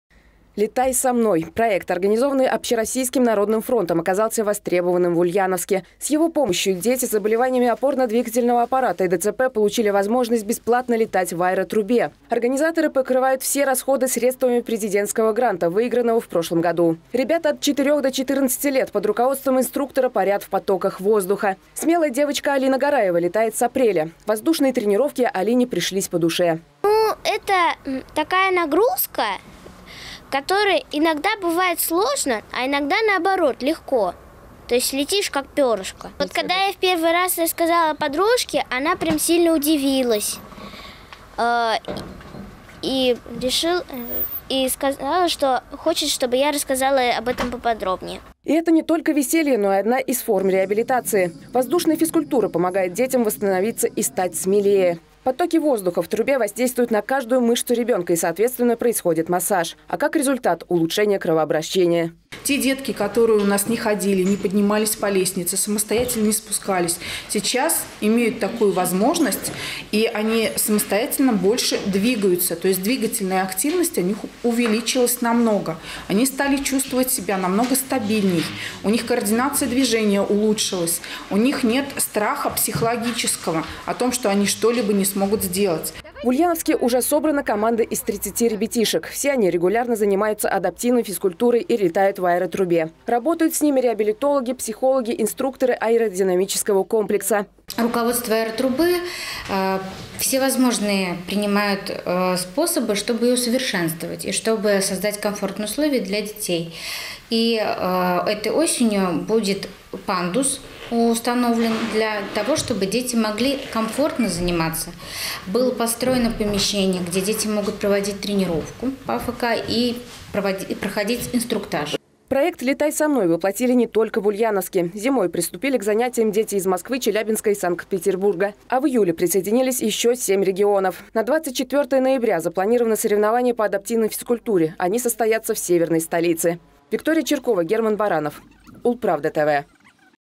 «Летай со мной», или Как ульяновские дети становятся смелее
«Летай со мной» - проект, организованный общероссийским народным фронтом, оказался востребованным в Ульяновске. С его помощью дети с заболеваниями опорно-двигательного аппарата и ДЦП получили возможность бесплатно летать в аэротрубе.